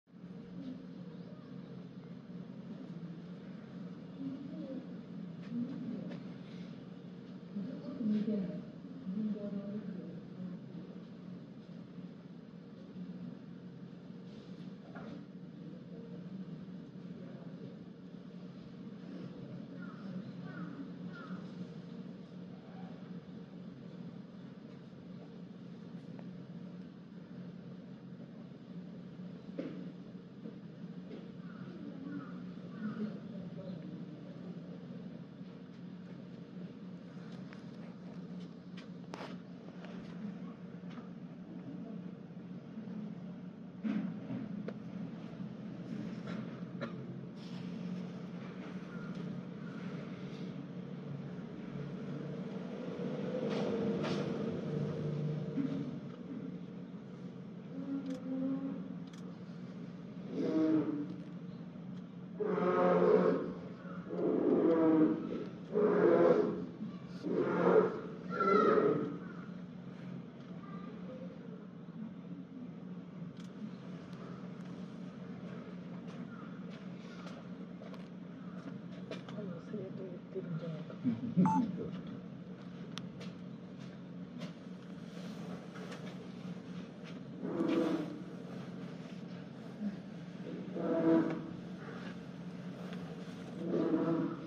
国内最高齢のみゆきちゃん🐻‍❄️お部屋の奥のほうでは、ゆめちゃんが夕飯をもらっていました🐻‍❄️🍖【王子動物園】（2023年12月23日）
夕飯の準備をする飼育員さんに、「早く欲しいでしゅ」と、ゆめちゃんが泣いていました。